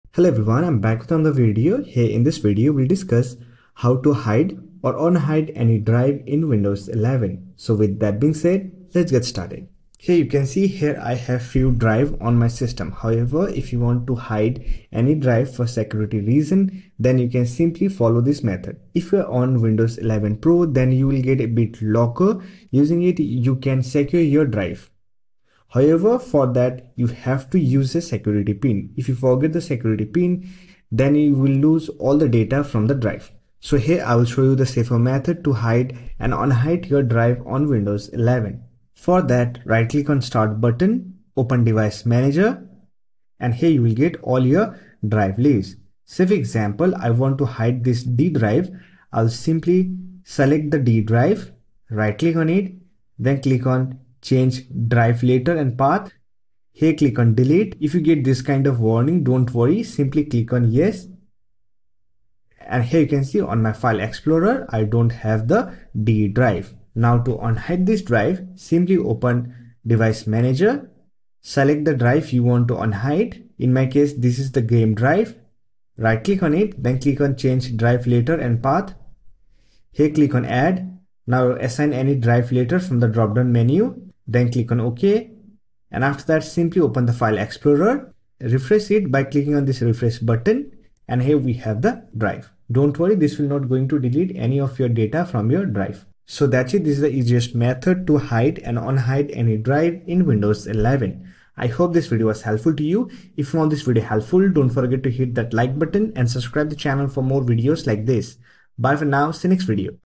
How to Hide or Unhide Any Drive in Windows 11
In this video, we are going to learn how to hide or unhide any drive in Windows 11. If, for security reasons, you want to hide any of your drives, you can easily do it using the Disk Management tool. Here's how.